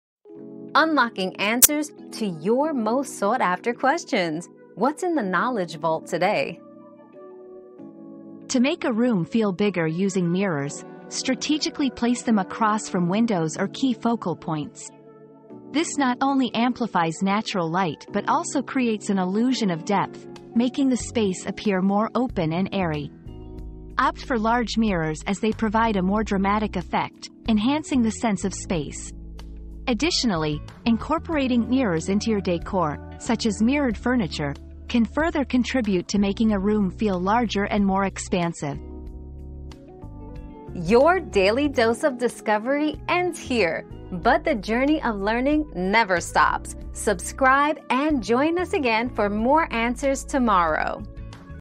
How do you make a room feel bigger with mirrors?
Mirrored Magic: Making Rooms Feel Bigger 👉 Mirrored Magic 👉 Learn how to create the illusion of extra space in your room by strategically placing mirrors to amplify light and depth. Discover the magic of large mirrors and mirrored decor in expanding your living space.

Laura S. Harris (2024, April 28.) How do you make a room feel bigger with mirrors?
    🌐 AskAbout.video